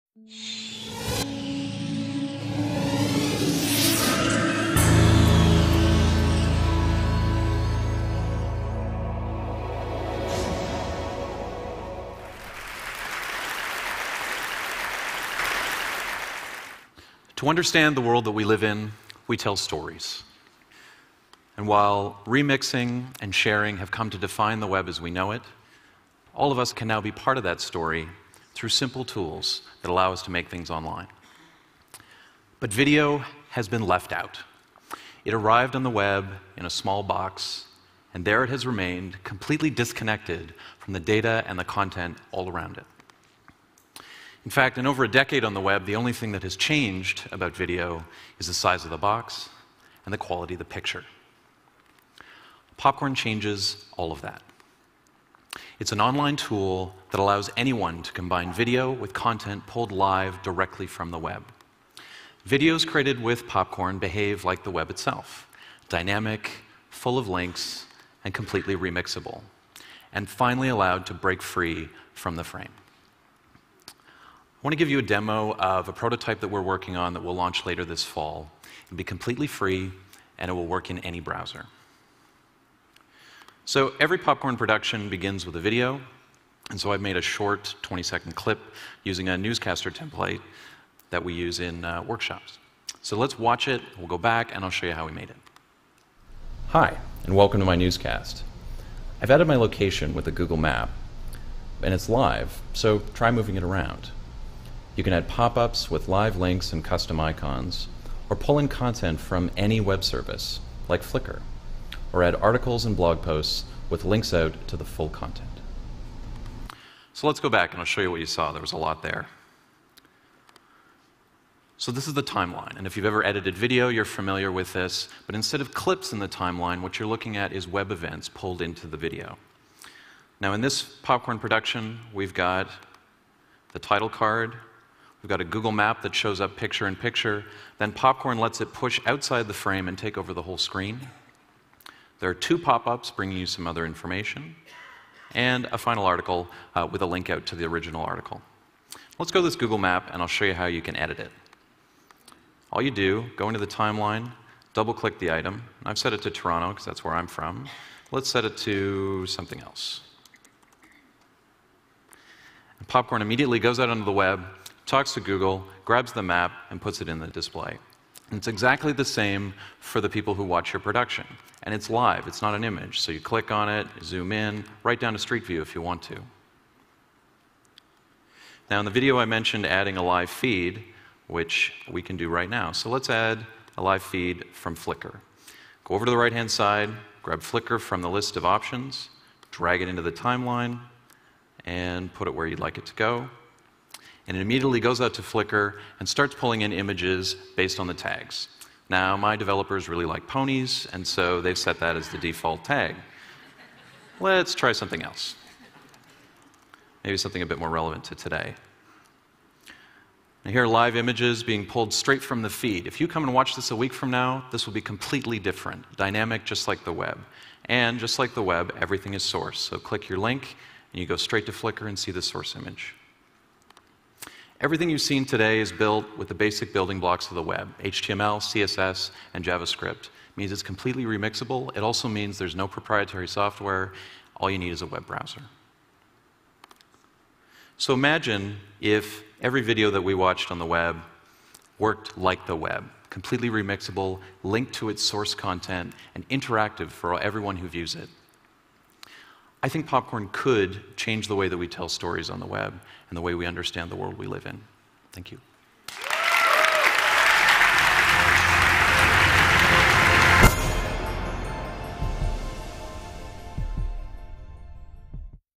Ryan Merkley: Online video -- annotated, remixed and popped
Videos on the web should work like the web itself: Dynamic, full of links, maps and information that can be edited and updated live. Mozilla Foundation COO Ryan Merkley demos Popcorn Maker, a new web-based tool for easy video remixing.